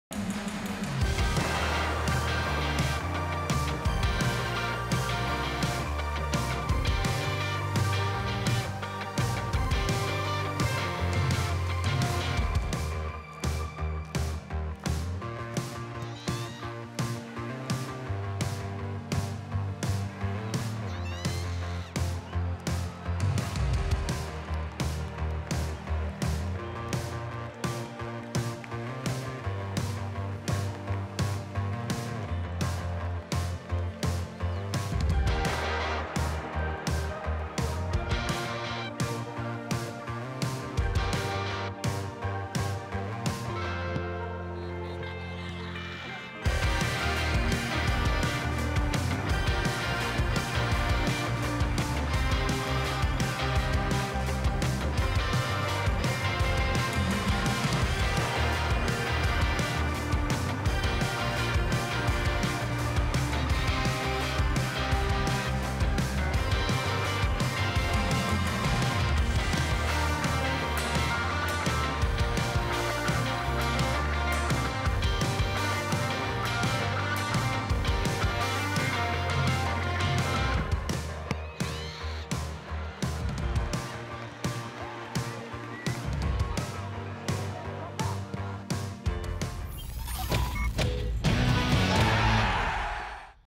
Jean Fernandes es el Tapador de la fecha 4 | Torneo Clausura Paraguay 2023
Con atajadas llenas de agilidad, Jean Fernandes dio seguridad al fondo de Cerro Porteño para encaminar el triunfo sobre Resistencia, llegando a su tercera valla invicta consecutiva en el torneo.
Mirá las mejores acciones del Tapador de la jornada 4 del Clausura.